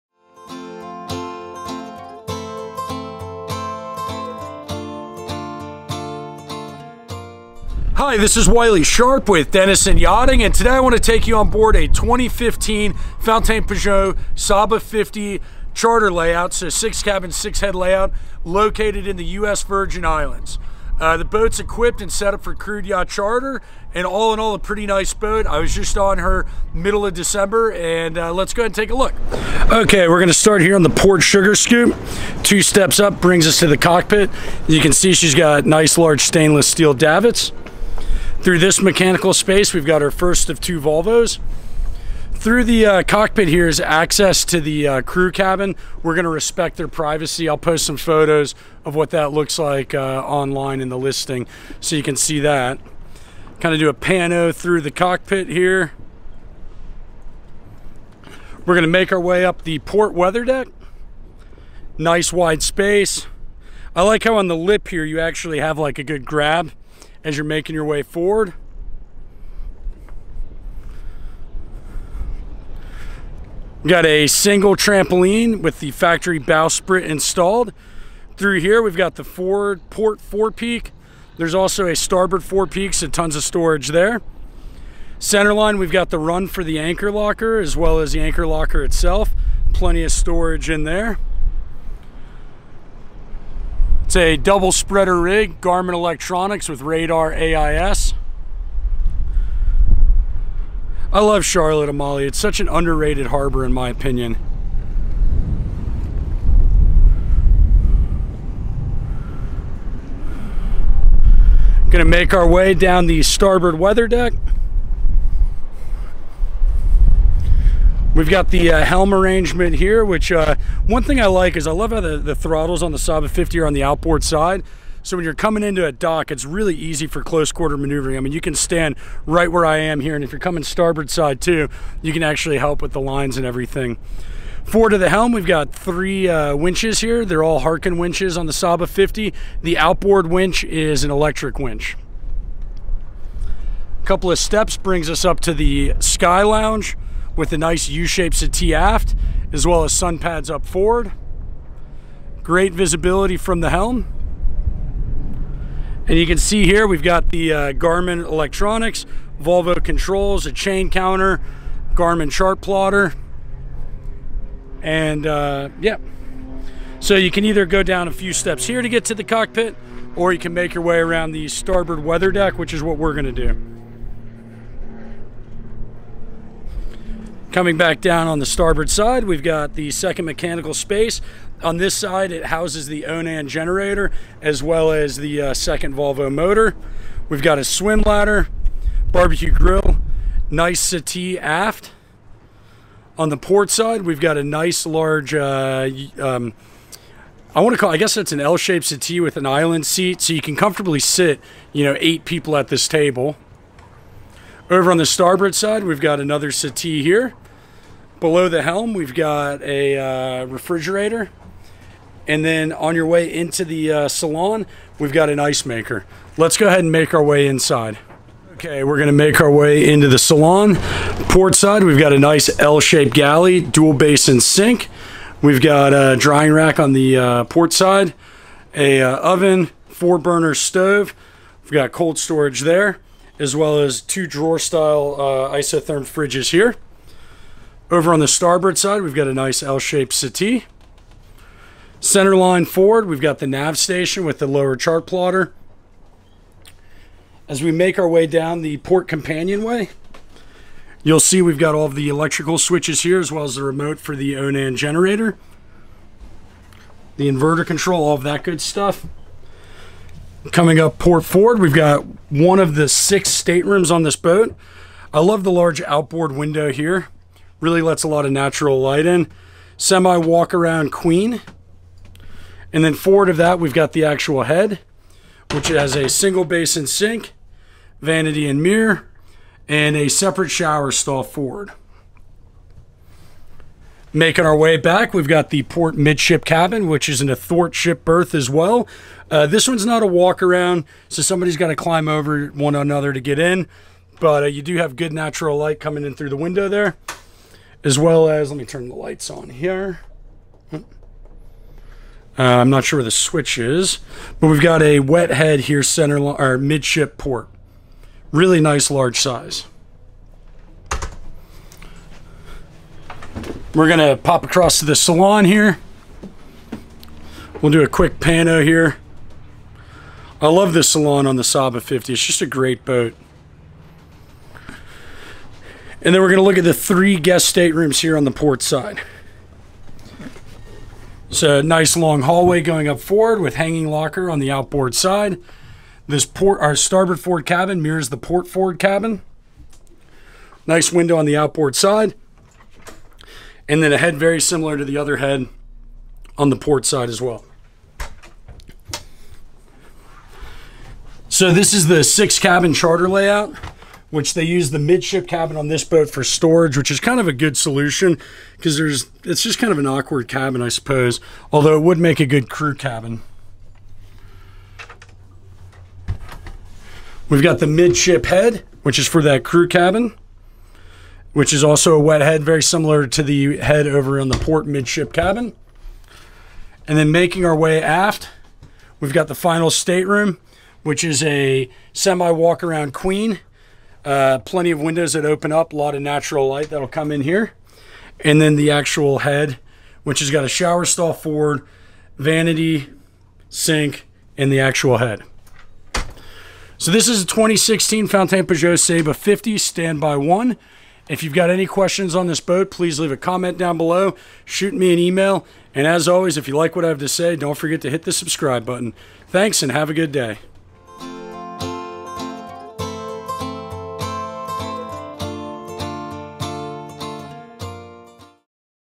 2015 Fountaine Pajot Saba 50 Walkthrough w/ Commentary [4K]
Stand By One is a 2015 Fountaine Pajot Saba 50 located in the Virgin Islands.  She is currently being used for crewed yacht charter, and can either remain in charter or be sailed away by her new owners.

If you have any interest in chartering her before you buy please check out the charter brochure on the boat:

Please don't hesitate to reach out if you have any questions on her, or any other catamaran.